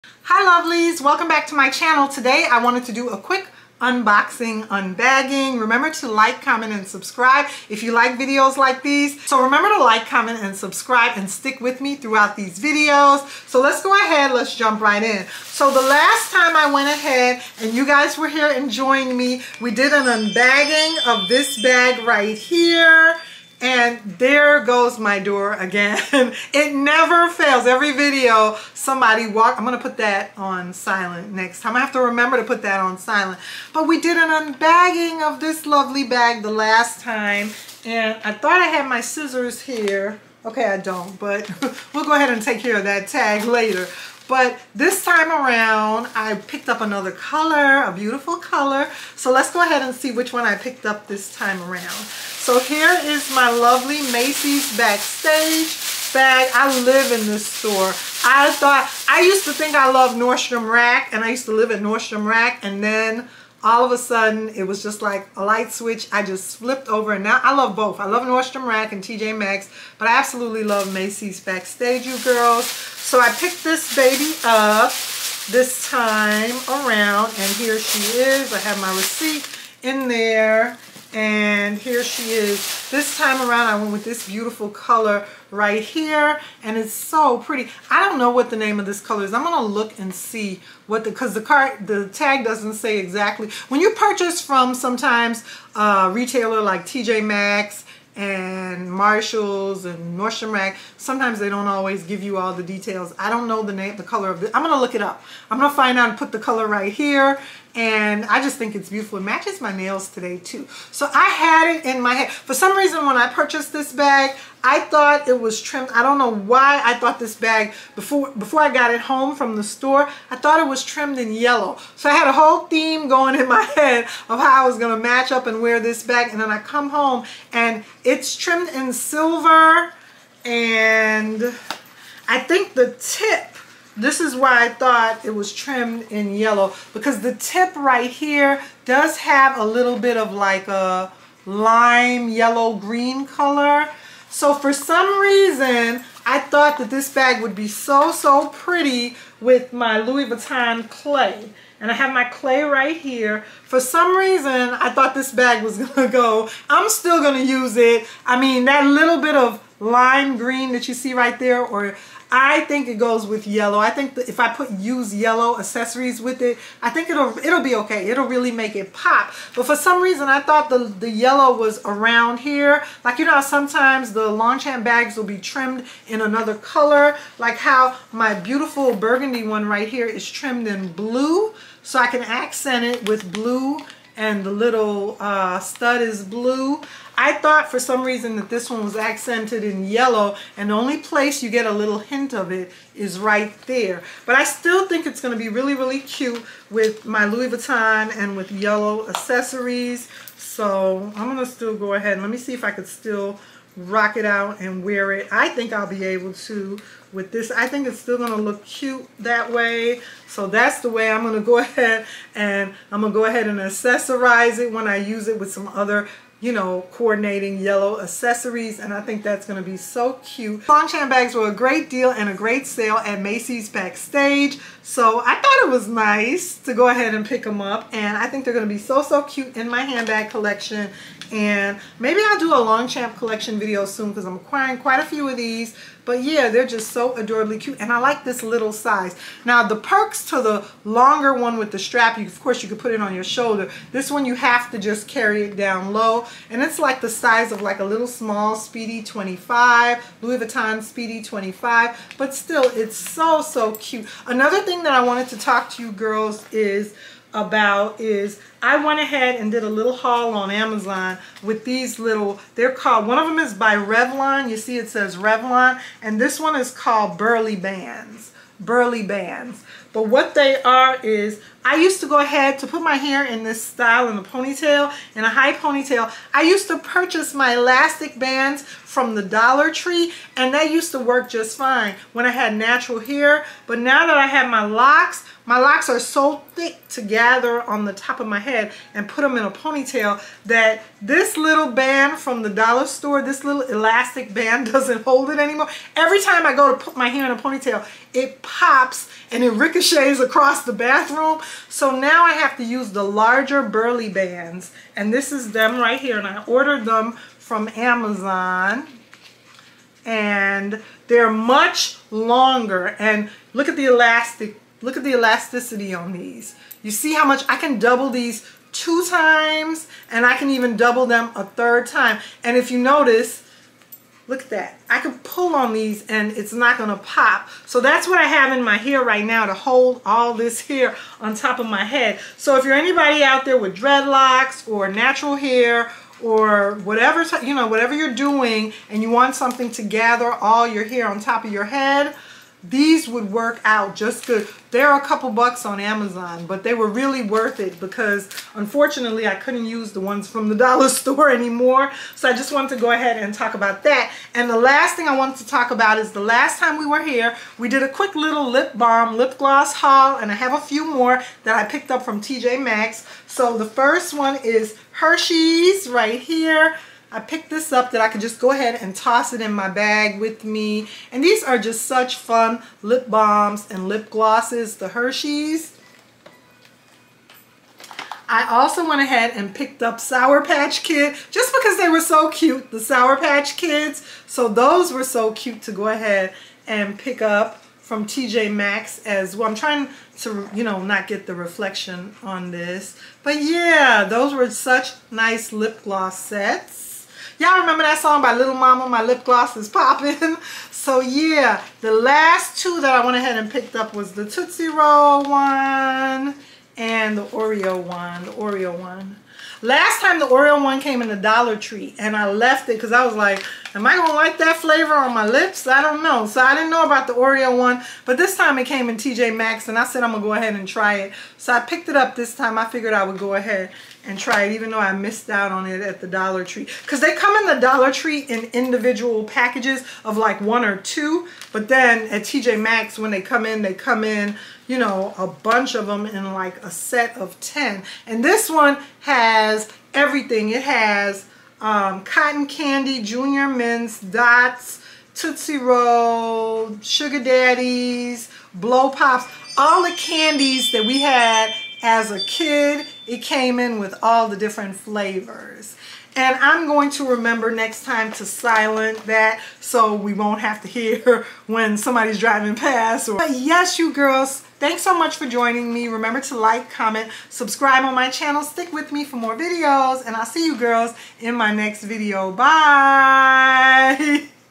Longchamp Bags "UNBELIEVABLE" Deal at Macy's Backstage *AND ANOTHER ONE*
Turn your everyday look into an extraordinary one with these amazing deals on Longchamp bags at Macy's Backstage!
"UNBELIEVABLE" Deal at Macy's Backstage You Won't Believe the Price of These Longchamp Bags!
You won't believe the price of these Longchamp bags!
Timecodes:
0:00 - Intro
0:20 - Longchamp
1:10 - Longchamp LePliage
1:21 - Longchamp Club bag
1:32 - Longchamp tote bag
1:43 - Longchamp mini tote
2:03 - Macys Backstage